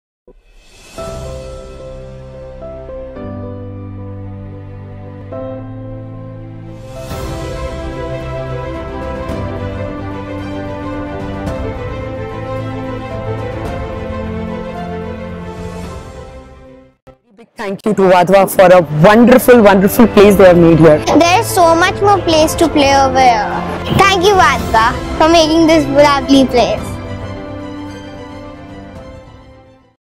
2 BHK Luxury Flats in Ghatkopar - The Address Ghatkopar
When you think 2 BHK luxury apartments in Ghatkopar, The Address in Ghatkopar West has got it all. With the perfect blend of cultures, we offer you community living at its best.